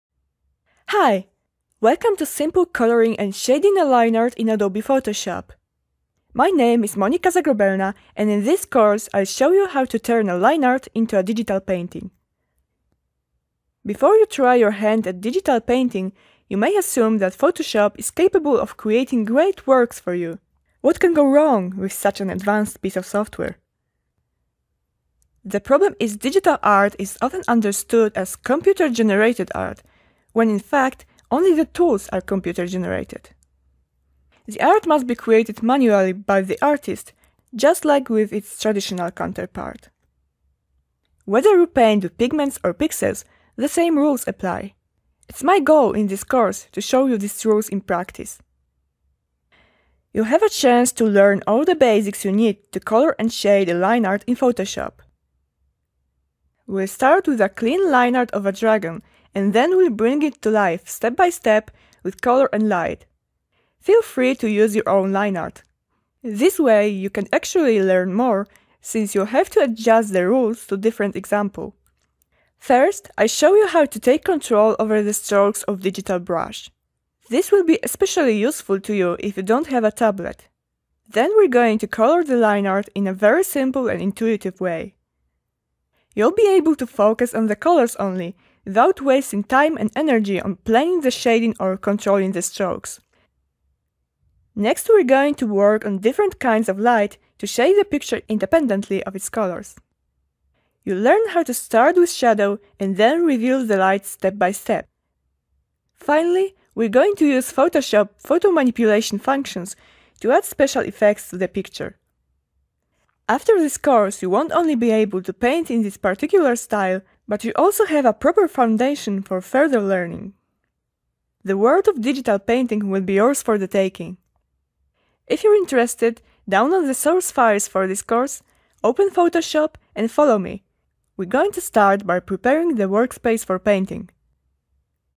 The Basics of Coloring and Shading Line Art in Adobe Photoshop: Introduction
Lesson 1 of 9, a Tuts+ course on The Basics of Coloring and Shading Line Art in Adobe Photoshop taught by Monika Zagrobelna. Become a Tuts+ member to get access to hundreds of new courses and eBooks!